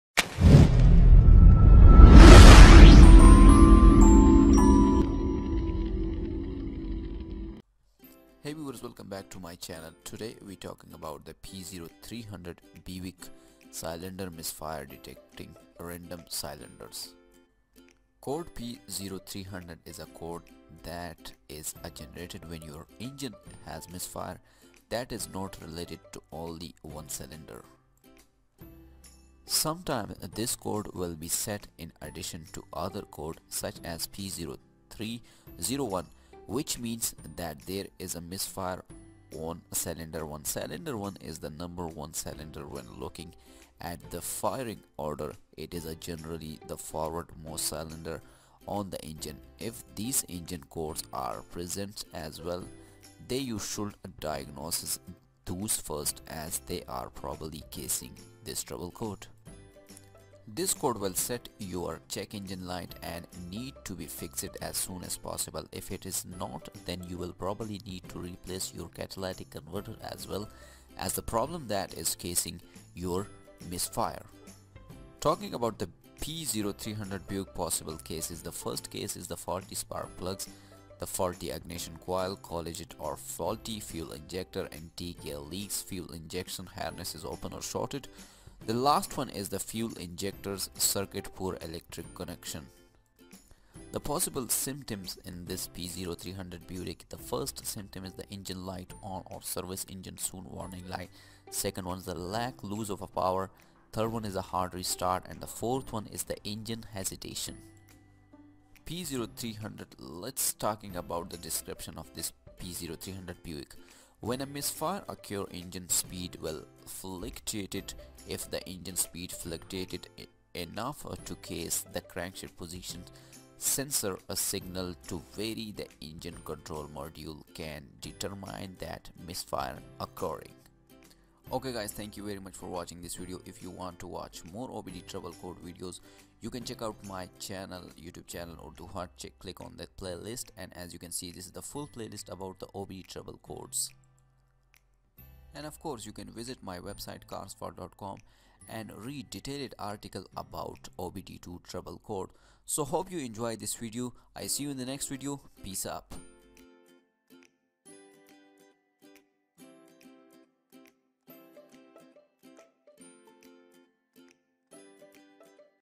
P0300 BUICK Cylinder Misfire Detected Random Cylinders
Possible causes
Faulty spark plug (s)
Faulty ignition coil (s)
Clogged or faulty fuel injector (s)
Intake air leak Fuel injectors harness is open or shorted
Fuel Injectors circuit poor electrical connection Ignition coils harness is open or shorted Ignition coils circuit poor electrical connection Insufficient cylinders compression Incorrect fuel pressure

or More Update Download and Install This App On Your Mobile :
مزید معلومات کے لیے یہ ایپ ڈائون لوڈ کر کیاپنے موبائل میں انسٹال کریں